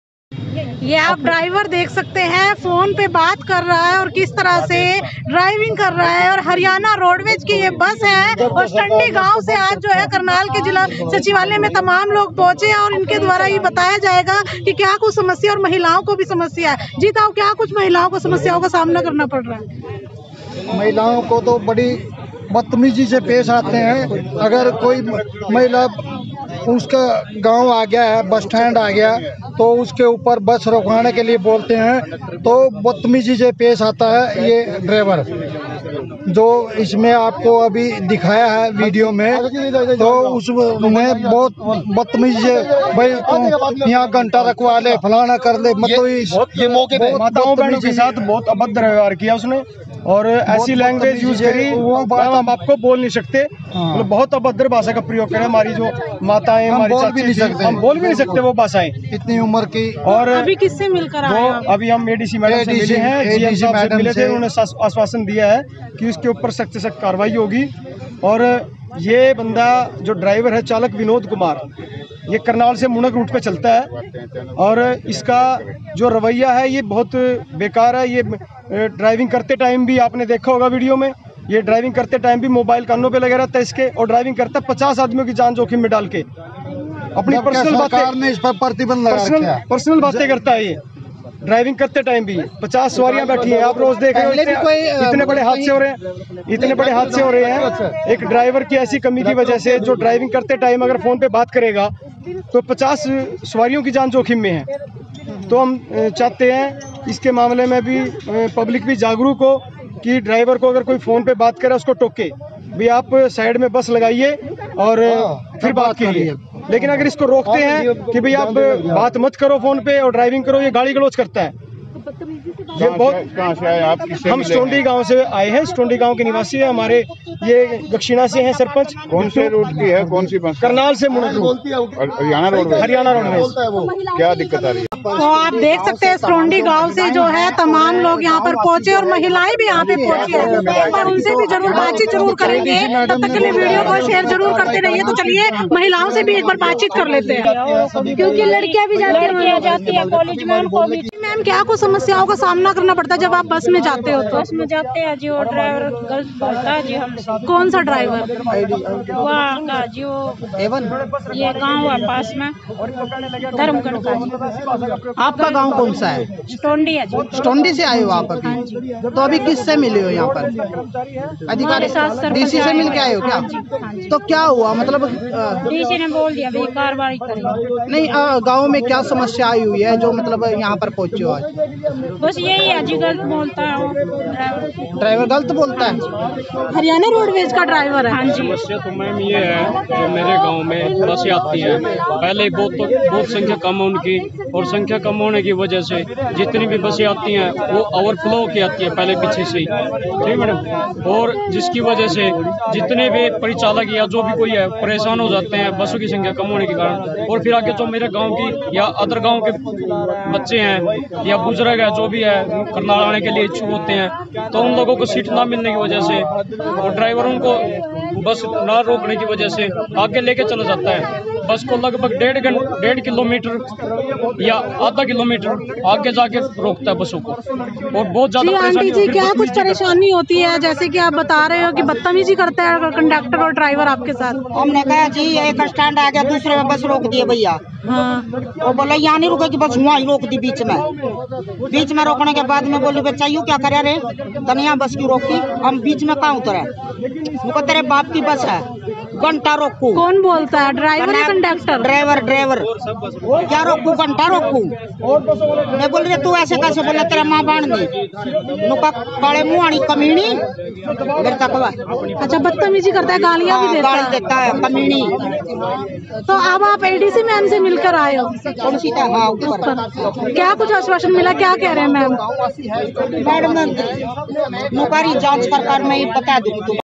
रोडवेज चालक ने महिलाओं से की बदतमीजी तो स्टोण्डी गांव वासी पहुंचे करनाल। Janta Ki Awaaj | Breaking
फेसबुक GROUP
GRAMIN BREAKING NEWS